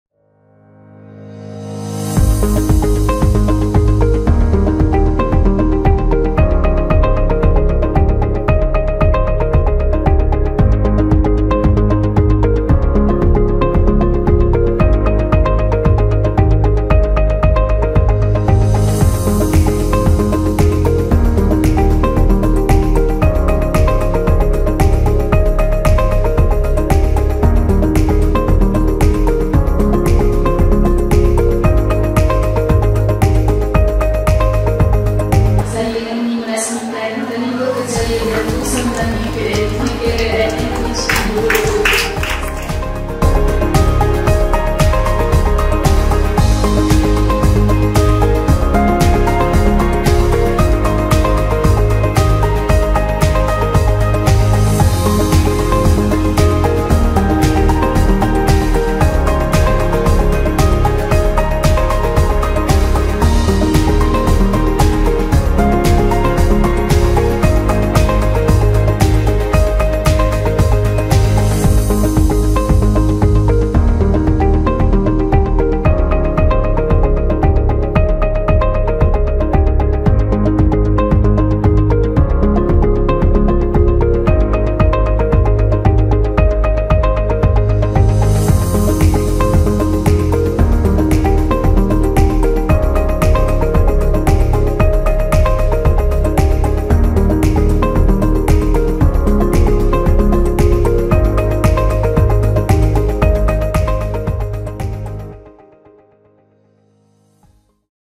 Sekitar Karnival Kerjaya dan Keusahawanan
Sekitar Karnival Kerjaya dan Keusahawanan yang berlangsung selama dua hari bermula pada 17 – 18 Oktober 2023 di sini menyaksikan penyertaan 20 majikan yang menawarkan 1,000 peluang kerjaya, selain siri bengkel dan seminar yang merangkumi pelbagai topik seperti perkongsian bersama syarikat, tips penampilan diri, penulisan resume berimpak tinggi dan petua temuduga.